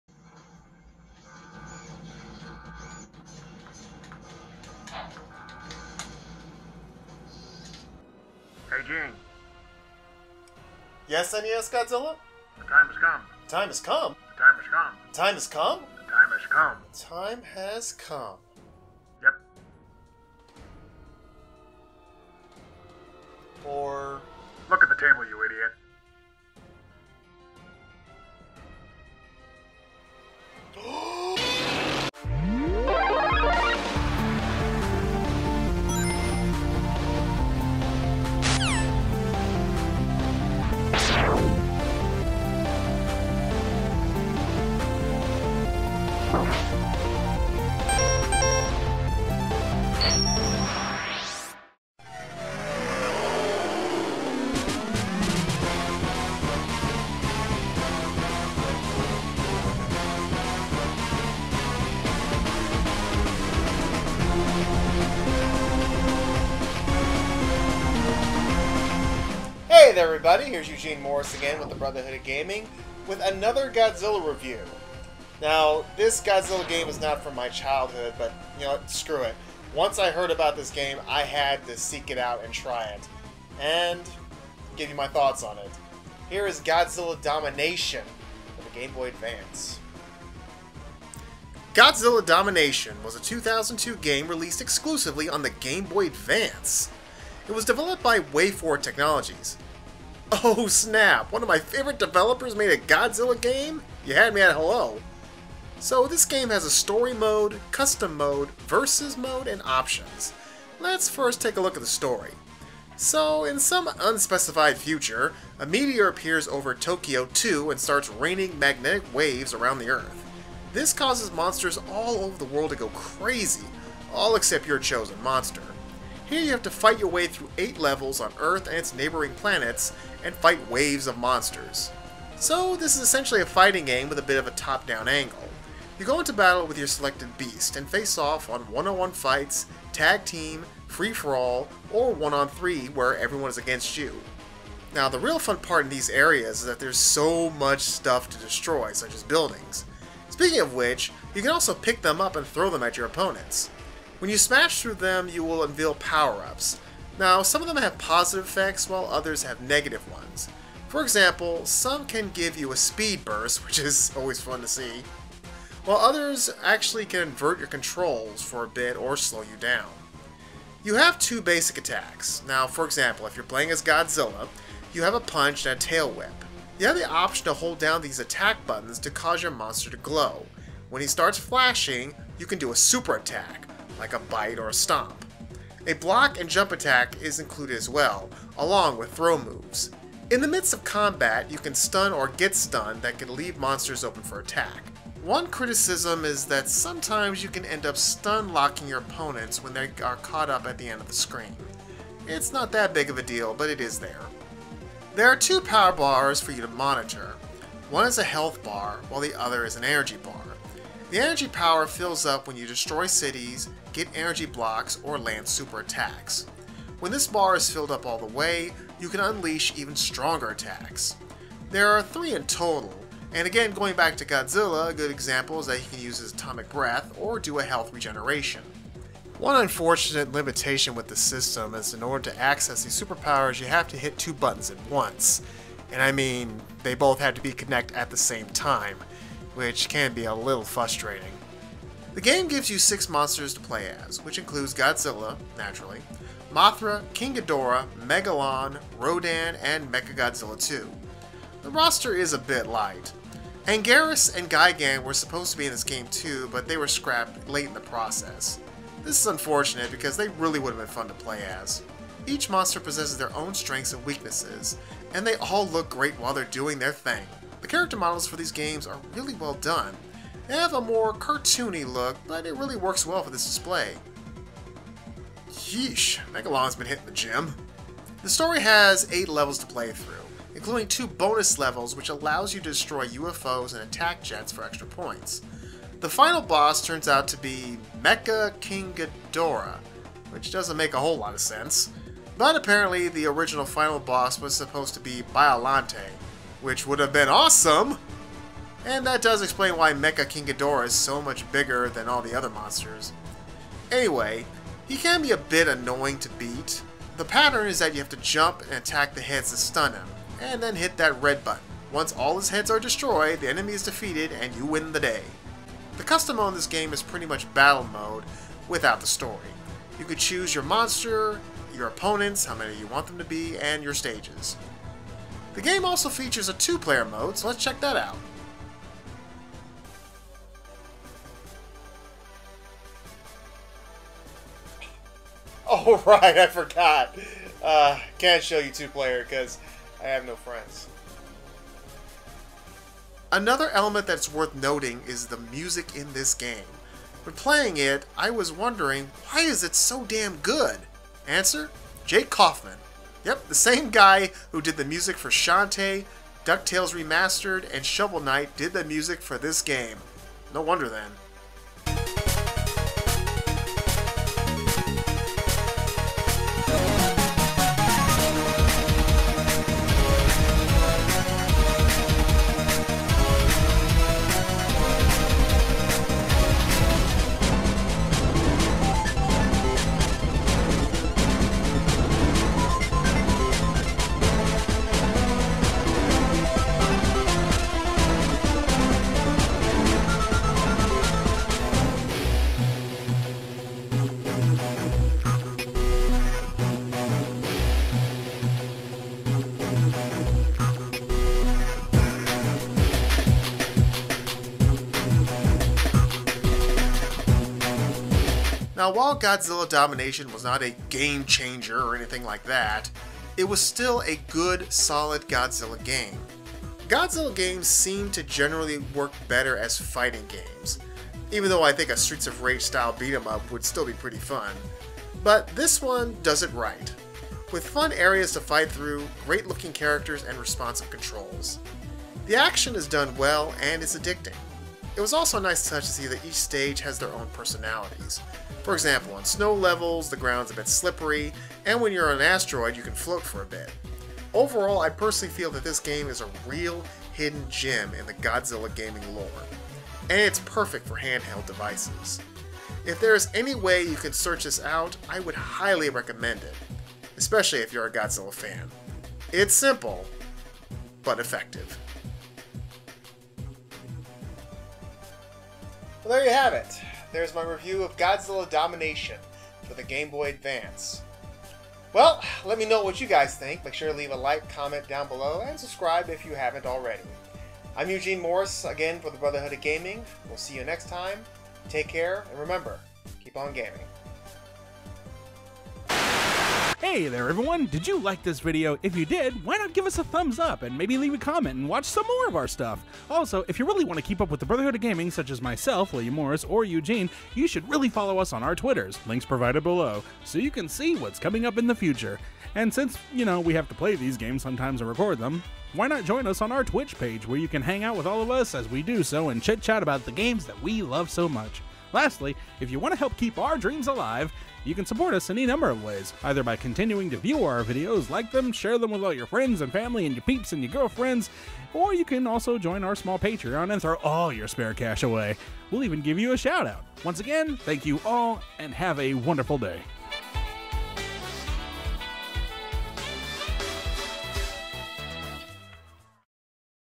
Godzilla: Domination! Review - The Brotherhood of Gaming
"One more Godzilla review before the new movie comes out!  This one was a hidden gem for the Gameboy Advance!  We are The Brotherhood of Gaming and this is our review of Godzilla: Domination!"